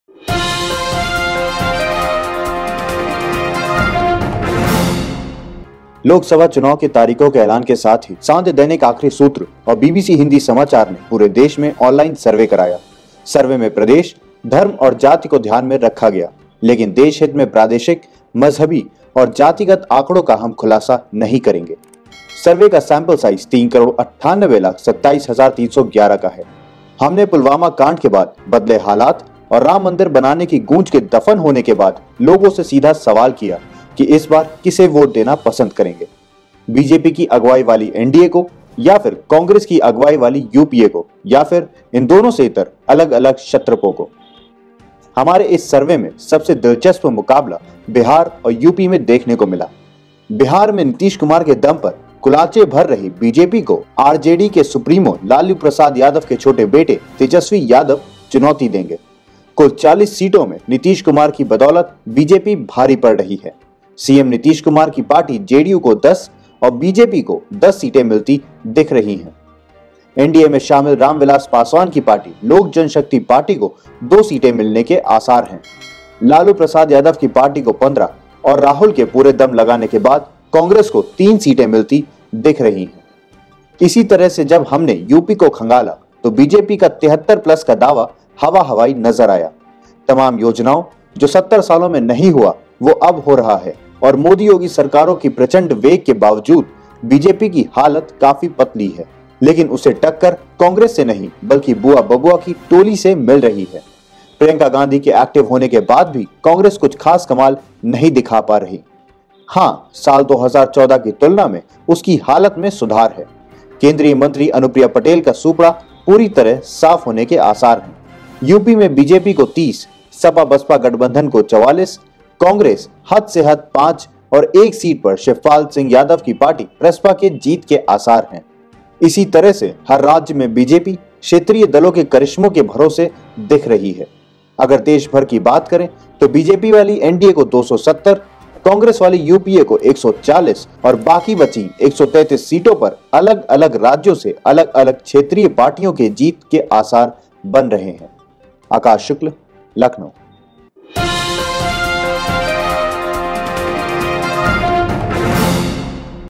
NDA सत्ता से बेदख़ल तो प्रधानमंत्री कौन ? | Opinion Poll News
लोकसभा के चुनावो की तारीखो का ऐलान हो चुका है , ऐसे में भाजपा की NDA और कांग्रेस की UPA दोनो ही जनता के साथ के साथ सत्ता का स्वाद नहीं चख पा रहे है । क्या मायावती और अखिलेश का महा गठबंधन इस बार के चुनावों में किंग मेकर बनकर उभरेगा ।